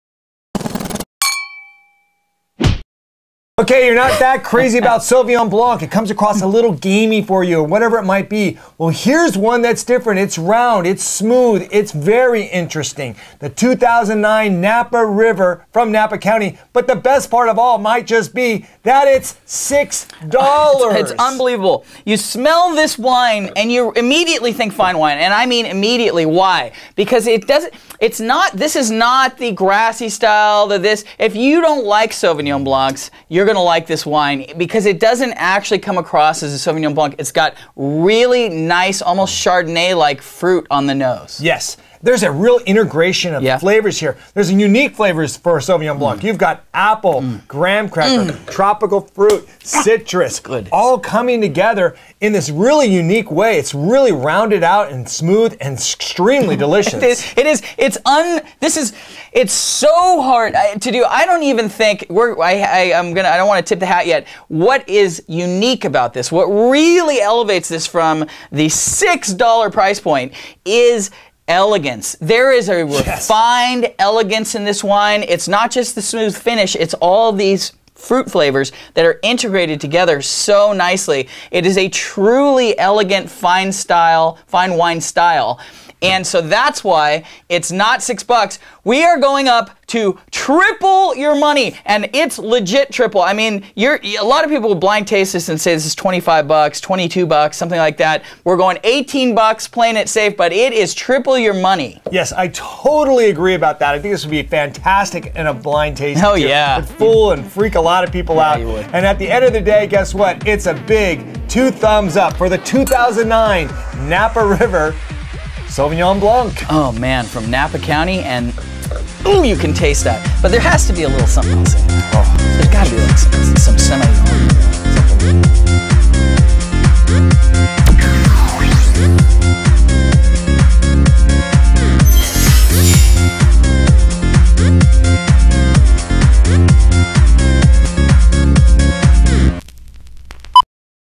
Thumbs Up Wine Review: 2009 Napa River Sauvignon Blanc, Two Thumbs Up
Wine Reviews by Thumbs Up Wine Review: Okay, so maybe you're not that crazy about Sauvignon Blancs.  Too grassy?  Too gamey?  Well here's one that's a little different.  The 2009 Sauvignon Blanc from Napa River is round, smooth and very interesting.  Almost Chardonnay-like fruit on the nose.  The best part of all might just be - it's only $6!  We're in "Triple-Your-Money" territory on this one.

Click below and download our free wine review app, and you'll always find the best bottles when you're shopping in the wine aisle: